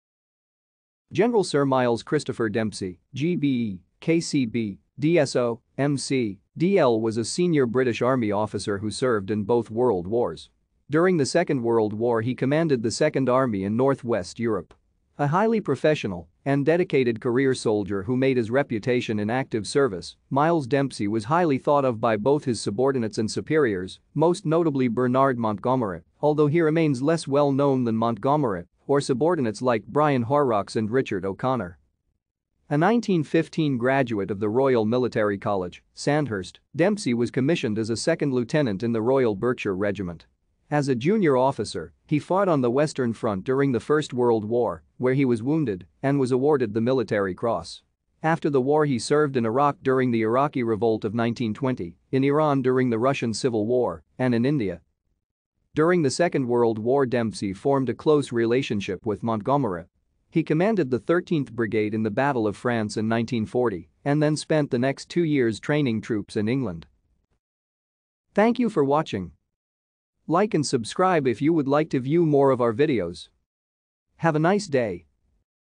Miles Dempsey
General Sir Miles Christopher Dempsey, GBE, KCB, DSO, MC, DL  was a senior British Army officer who served in both world wars. During the Second World War he commanded the Second Army in north west Europe. A highly professional and dedicated career soldier who made his reputation in active service, Miles Dempsey was highly thought of by both his subordinates and superiors, most notably Bernard Montgomery, although he remains less well known than Montgomery or subordinates like Brian Horrocks and Richard O'Connor.